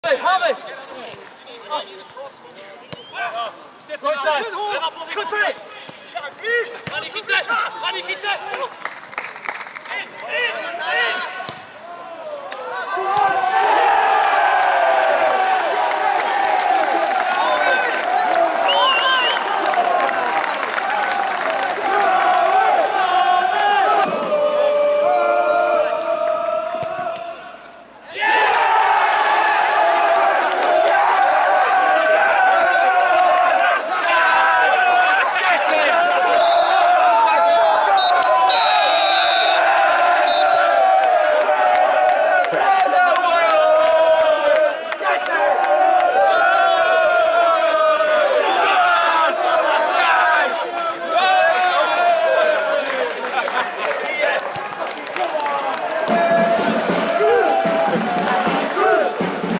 Fleetwood Town 2-1 FK Jablonec | Match Highlights
Watch both Town goals from Ashley Hunter and Joe Maguire from our win over Czech Premier Division side FK Jablonec at FC Kossen.